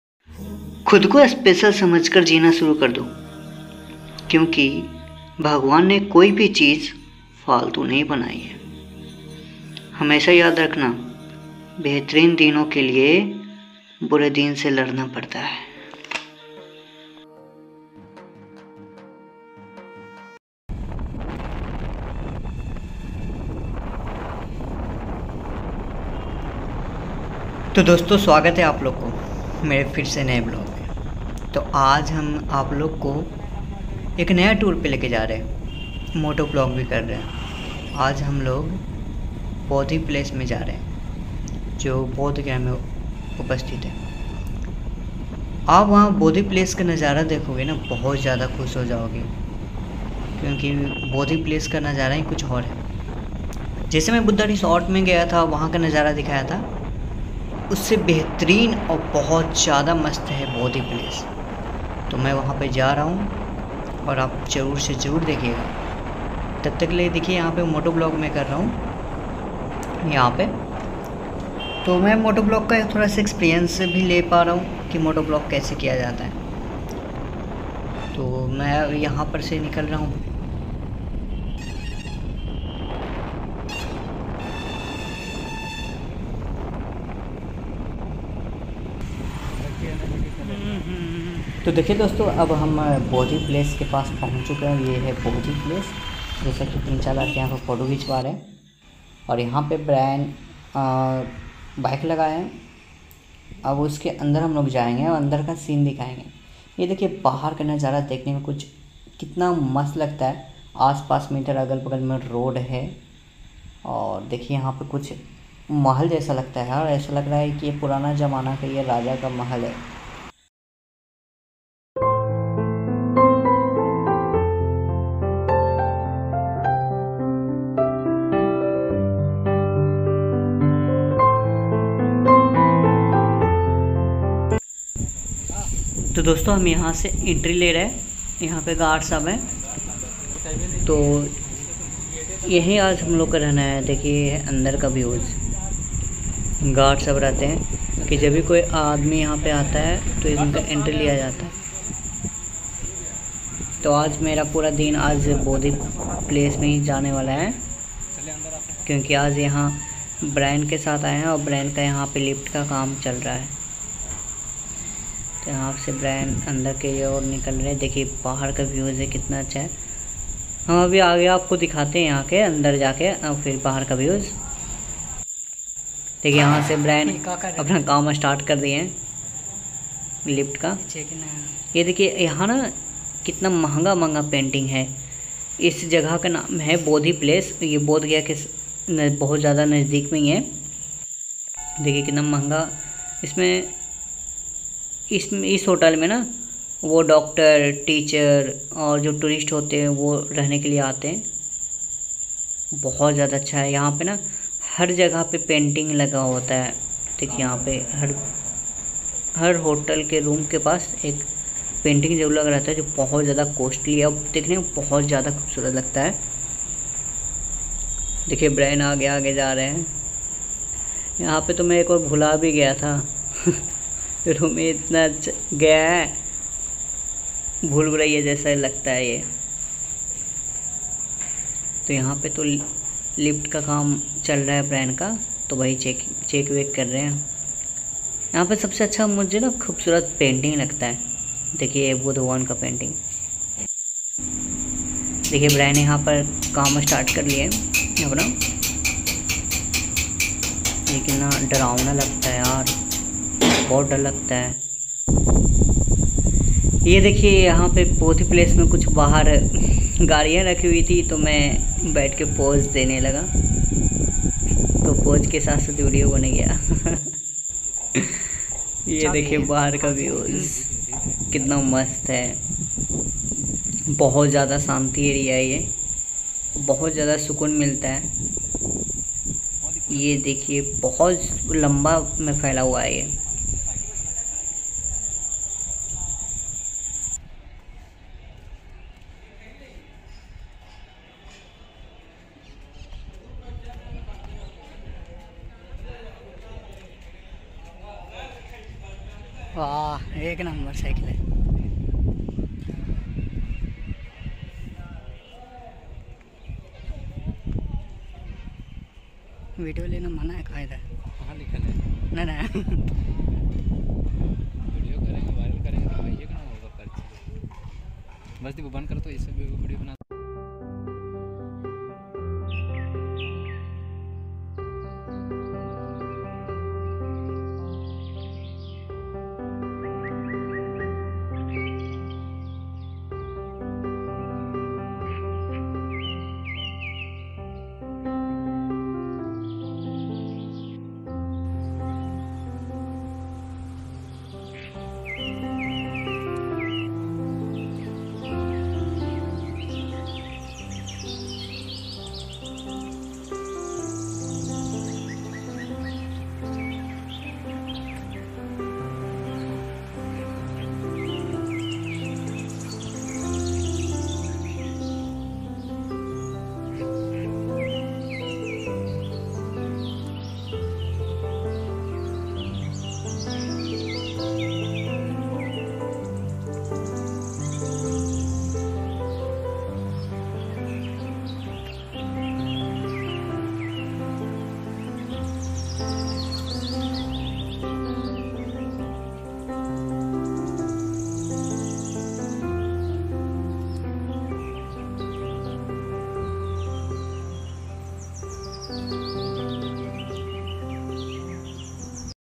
Vlog37 || Bodhgaya Bodhi Place Moto Vlog || Best Hotel In The Bodhgaya
Hello dosto iss vlog mein mai moto vlog kiya hun jo bodhayagay to bodhi place gaya jisme Mai bodhi place ke full review dala hun jo aapko bahut jyada pasand ayega ..
agar aapko ye video pasand aaya hoga to mere video ko like jarur kar kar de aur kuch bolna hai to mere comment section mein comment jarur kare mujhe aaplog ki comment ka wait rahta hai..